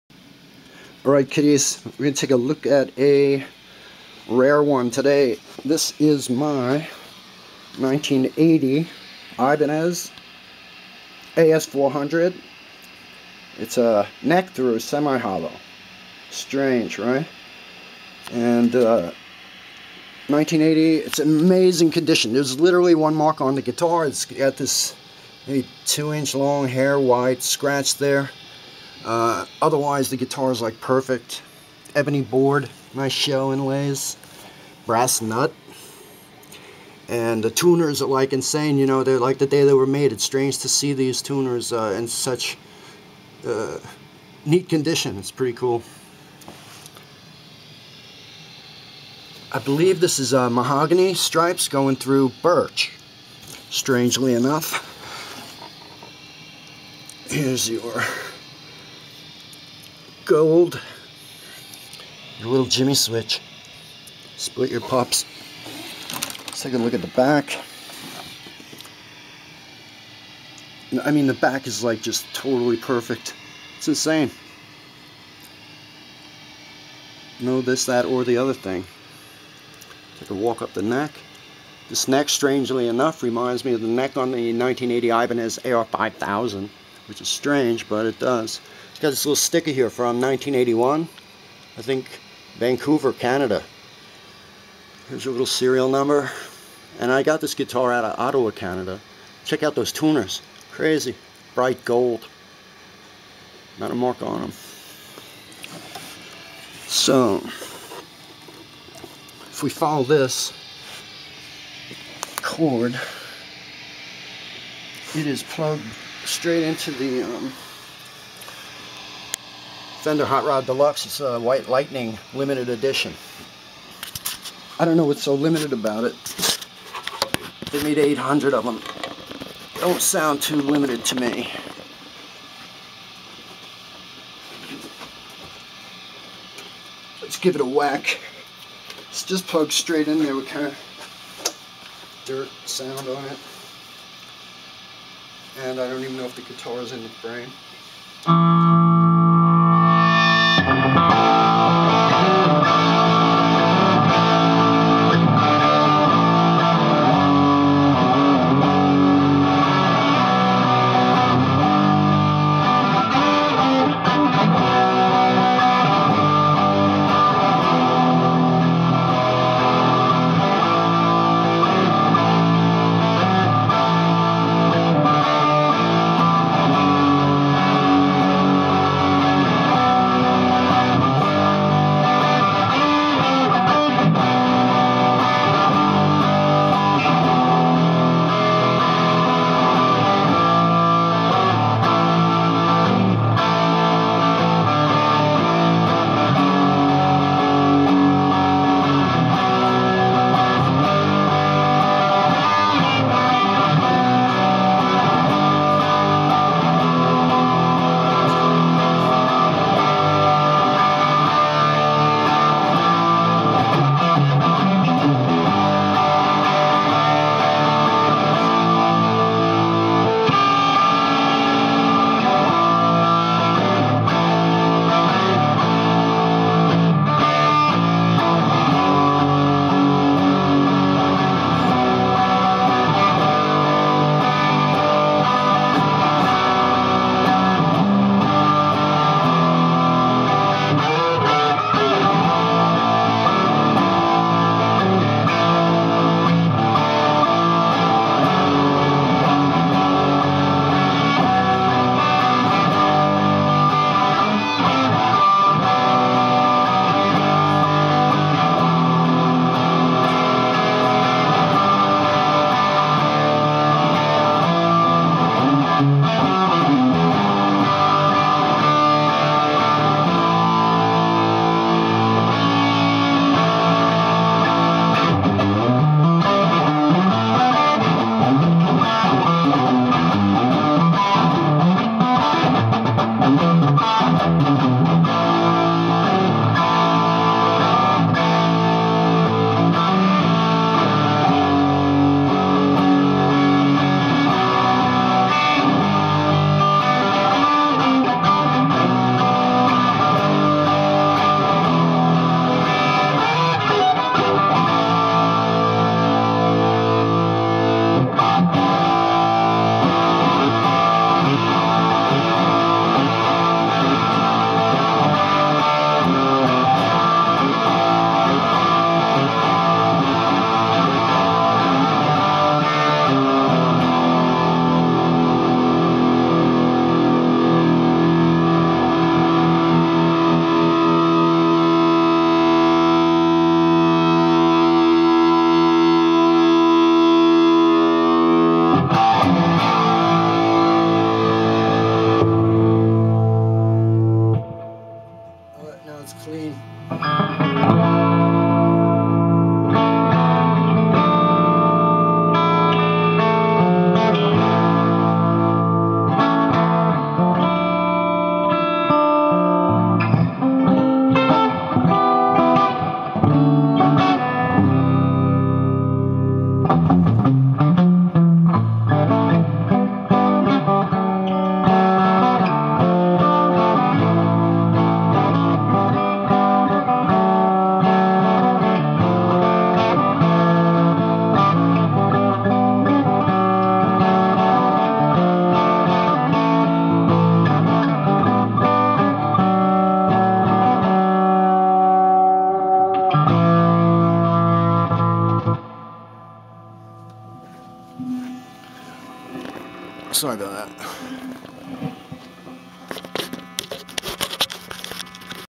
ibanez as400 semi-hollow neck-thru electric guitar rare 1980
1980 ibanez as400 semi-hollow neck thru electric guitar.
played extremely poorly.

154 of these ever made.
amazing guitar.